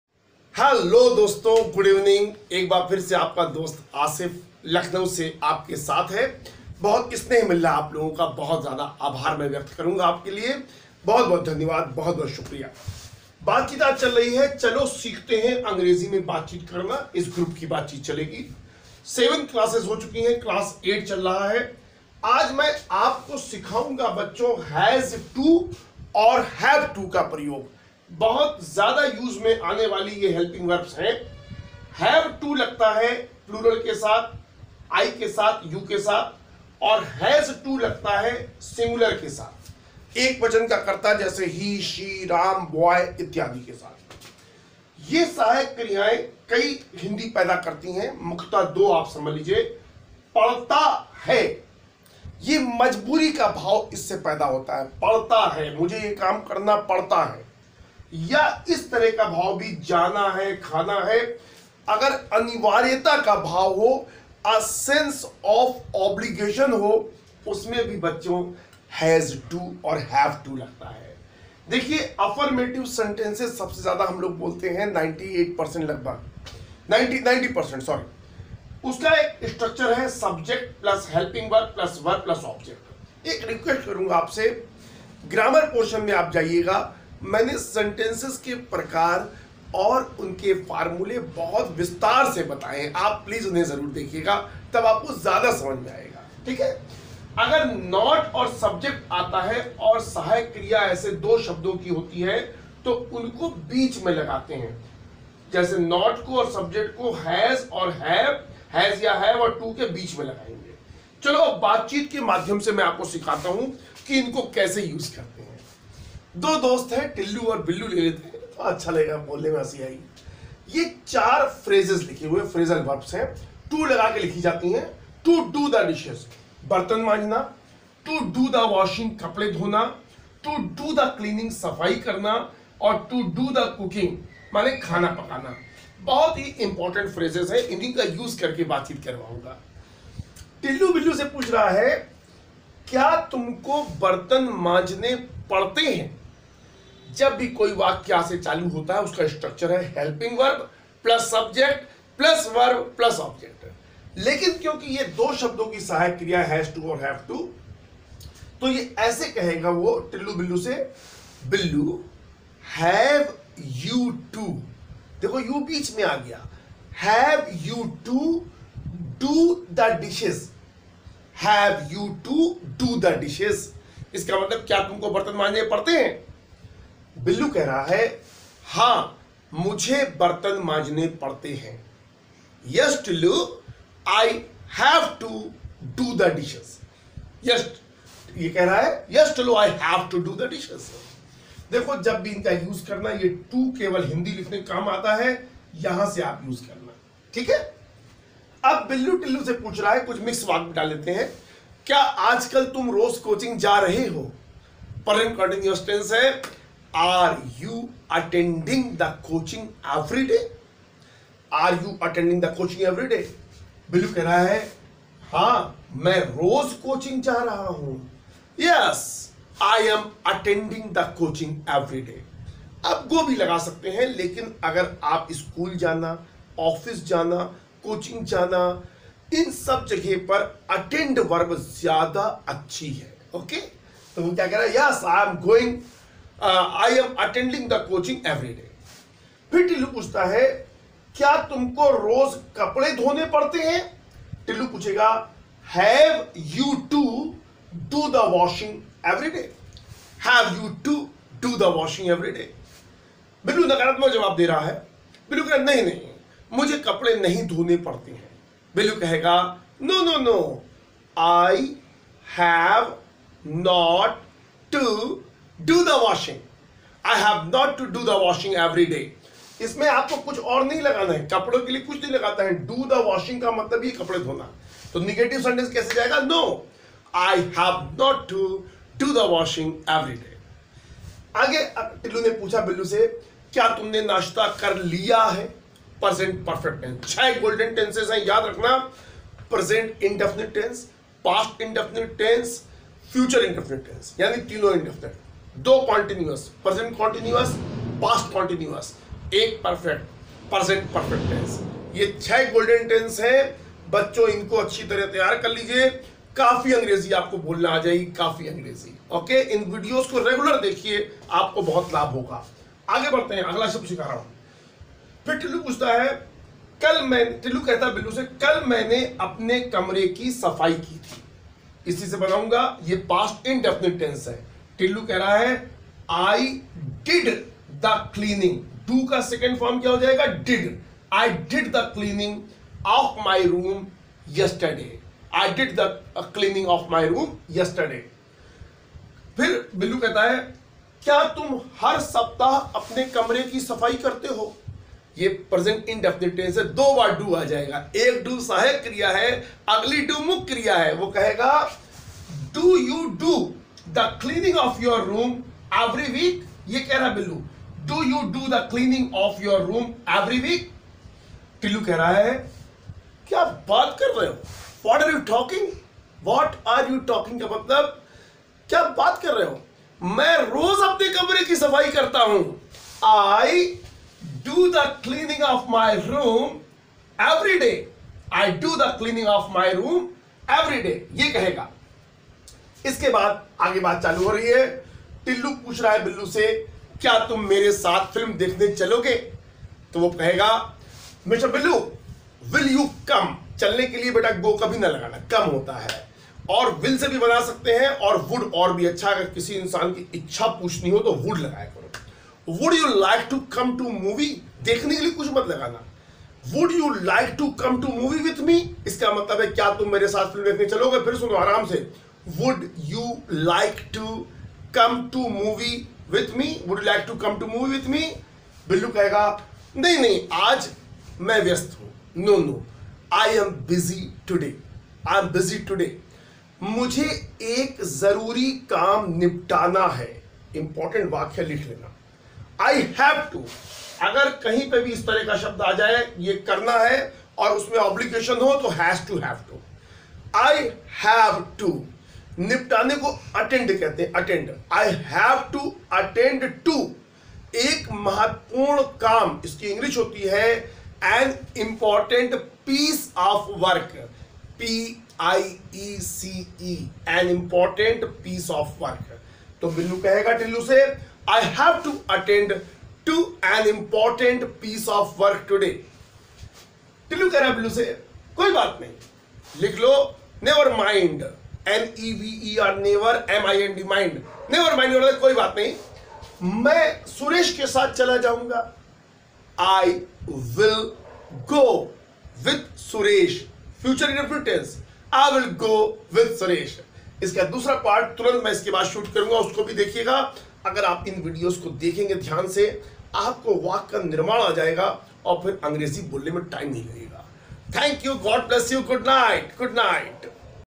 चलो सीखते हैं अंग्रेज़ी में बातचीत करना, Lecture 8
दोस्तों, इस वीडियो में दो लोगों की बातचीत के माध्यम से आपको स्पोकेन इंगलिश की ट्रेनिंग दी गई है।मिश्रित वाक्यों के साथ साथ आपको has to / have to वाले वाक्य भी सिखाये गए हैं।
आप इस ग्रुप के वीडिओज़ को ध्यान से देखिये, फिर इसकी मदद से खूब इंगलिश बोलिये, आपको जल्दी ही इंग्लिश बोलना आ जायेगी।
Thnx। आसिफ